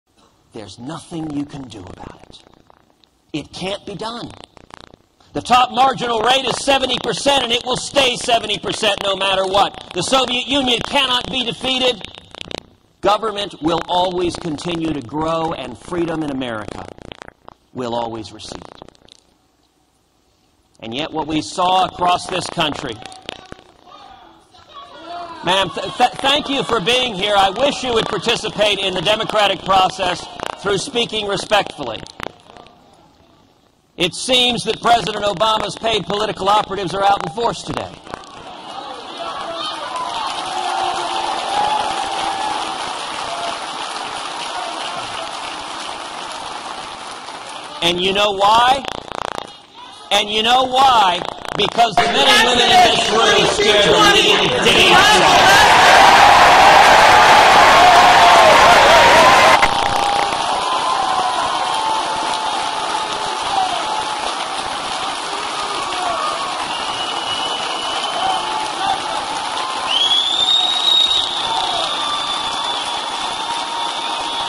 [RAW VIDEO] Ted Cruz deals with hecklers at Values Voter Summit
Watch Ted Cruz handle liberal hecklers at the FRC summit